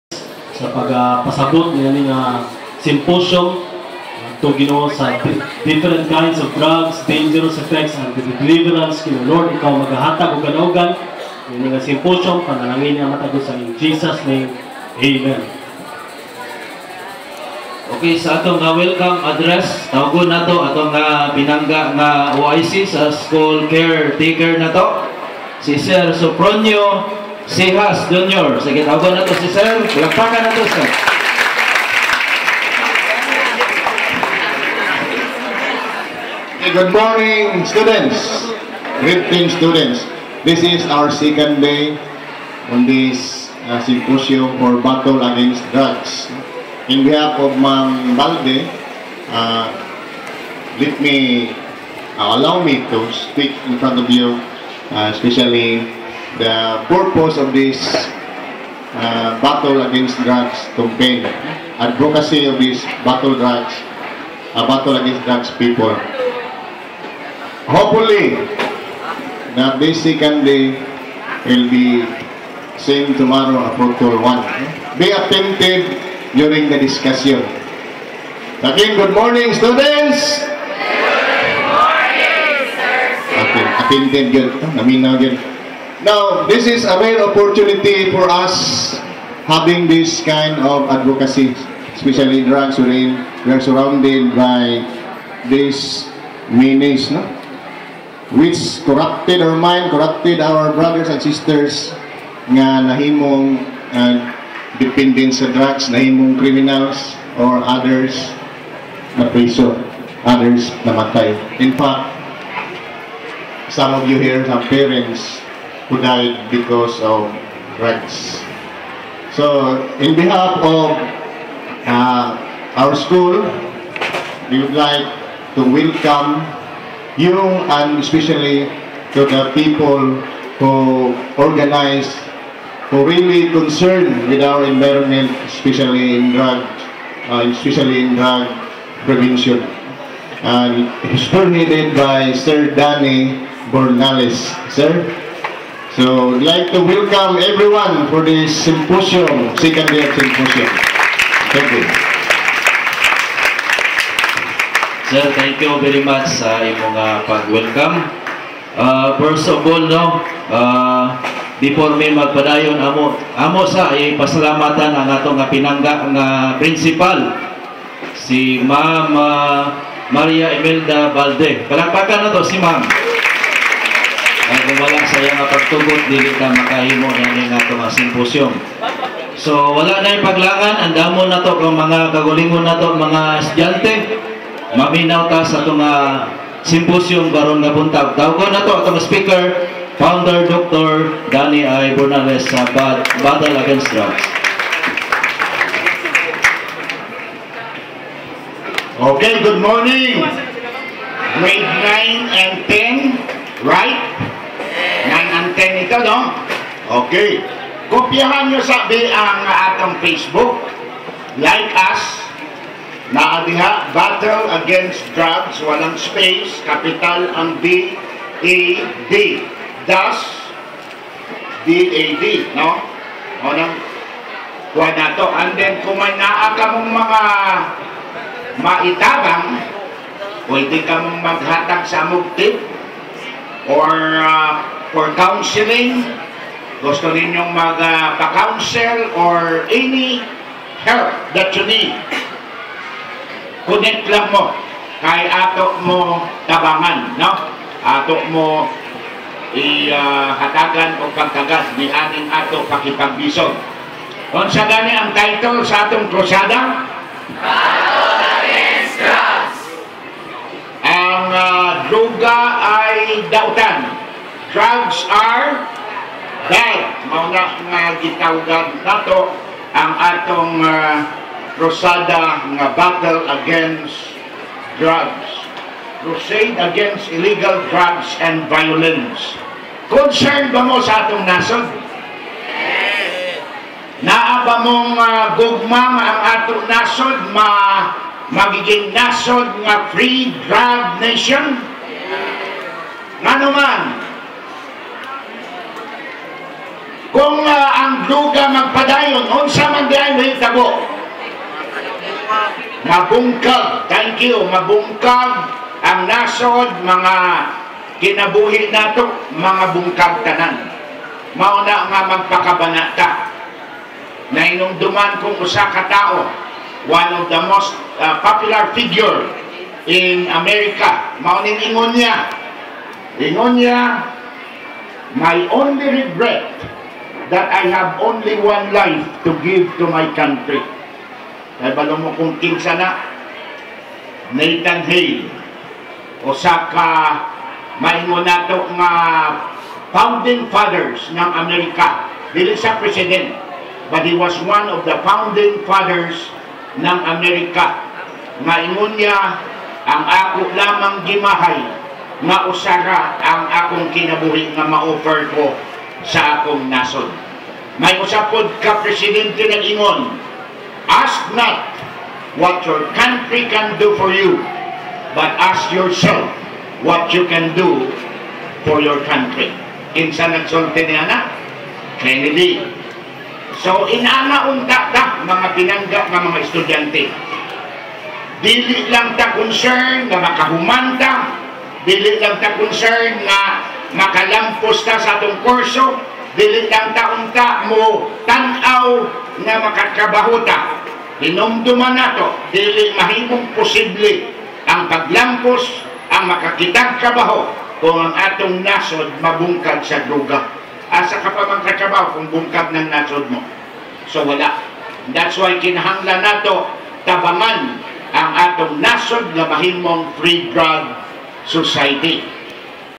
B.A.D at Cansojong NHS Gr 9&10 Welcome Remarks 7/14/17am
Speaker:

Dr Danny Bornales: Founder/Chairman of Battle Against Drugs

July 14 2017 am